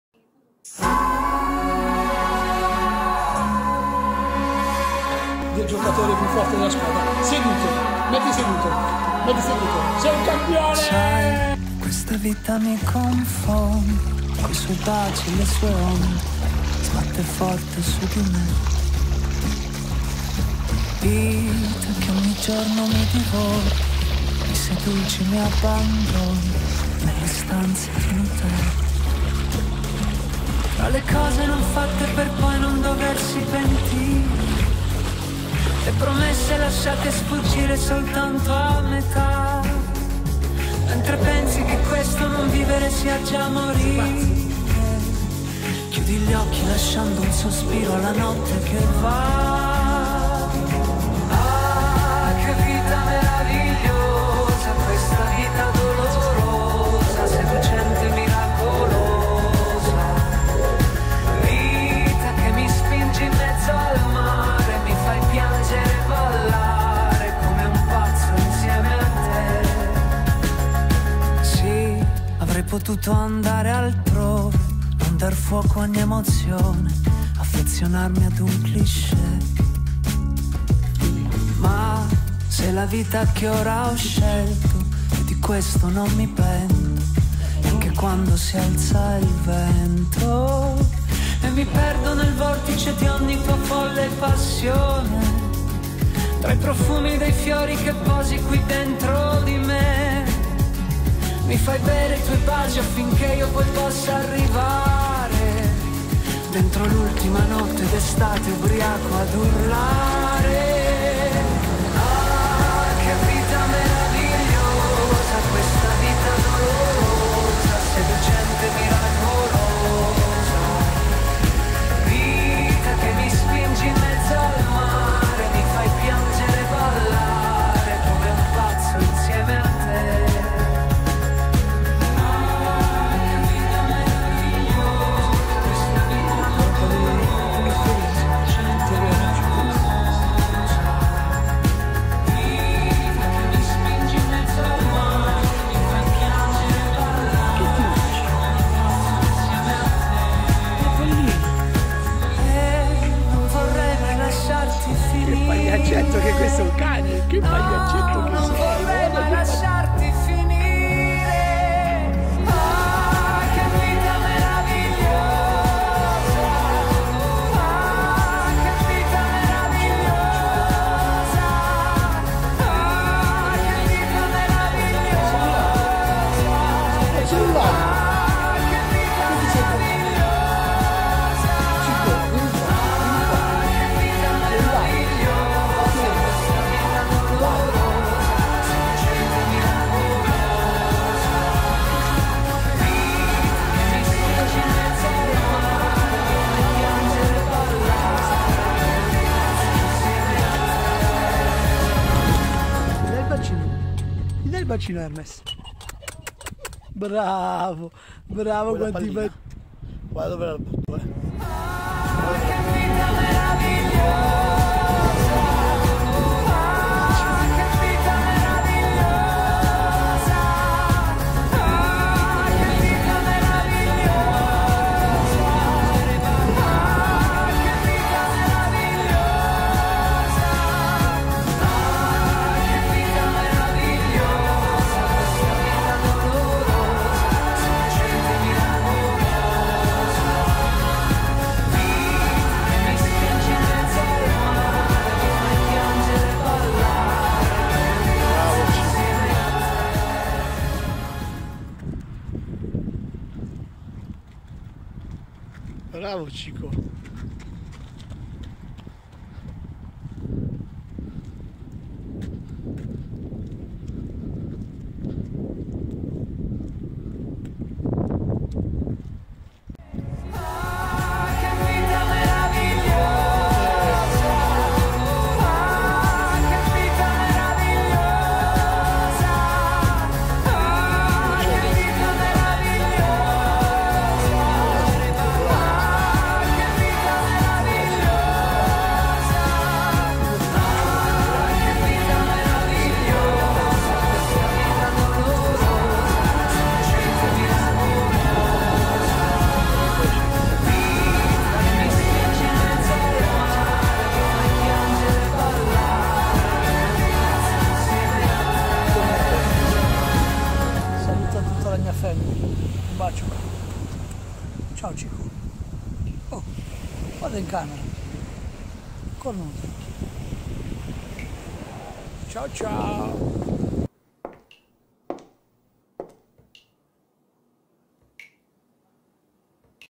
La giovinezza di Cico - Evoluzione e crescita di un Weimaraner "Mangià e durm"
La giovinezza di Cico, trascorre nel calore selvaggio di Santa Maria del Cedro, tra fiumi, tartufi, mare e corse nel parco nazionale del Pollino. In quelle pigre giornate autunnali felicità e spontaneità ci portano a spasso spensierati e sfiniti!
Dopo il successo di "Cico cerca moglie", il secondo episodio "la giovinezza di Cico" aggiorna la crescita e l'evoluzione del "Cavaliere dei due mari".

S1E1:
Cico cerca moglie